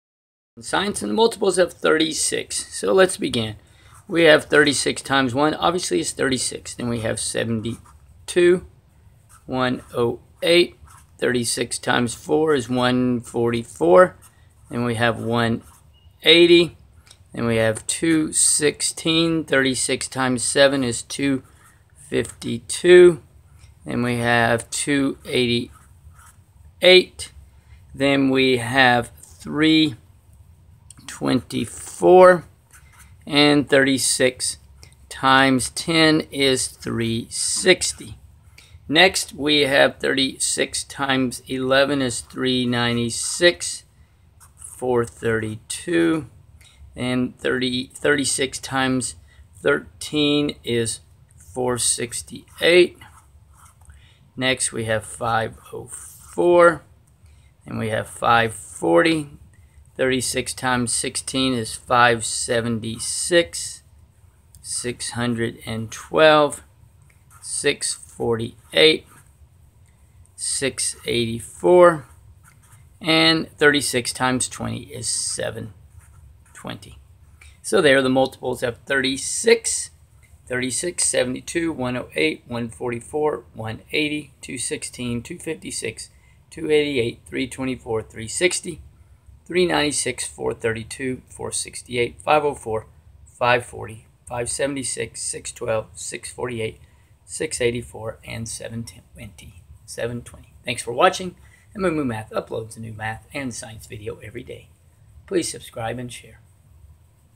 Multiples of 36
Learn about the multiples of 36.
The multiples of 36 are 36,72,108,144,180,216,252,288,324,360